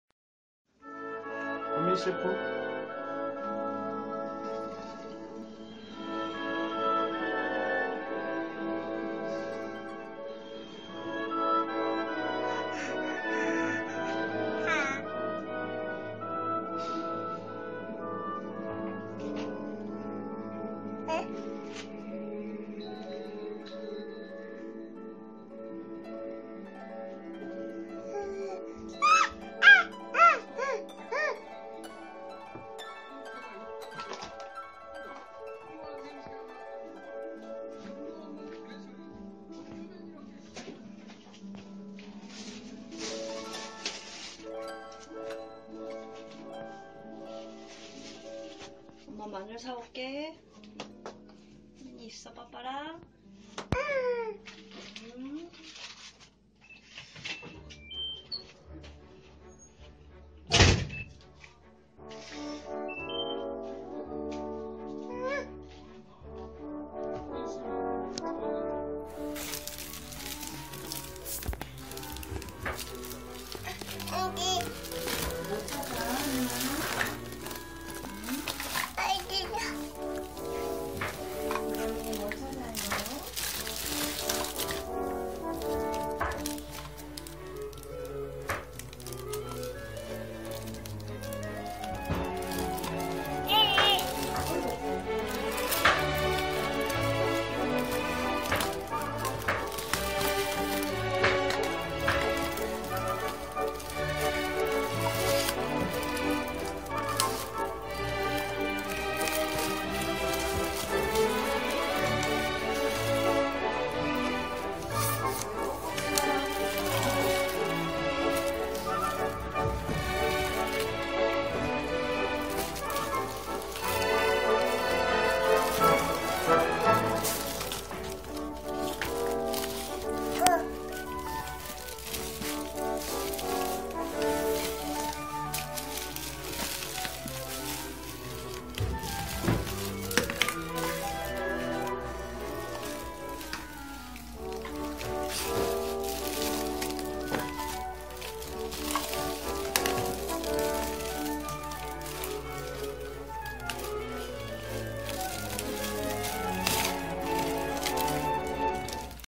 쉐프가 된 뚱자ㅎㅎ (아이스크림 보냉팩 반응)
아이스크림 담는 봉지 쓴 뚱자 쉐프가 됐네요 그 반응이 좋아 담아봤습니다 이 아이스크림보냉팩은 놀잇감으로 훌룡하네요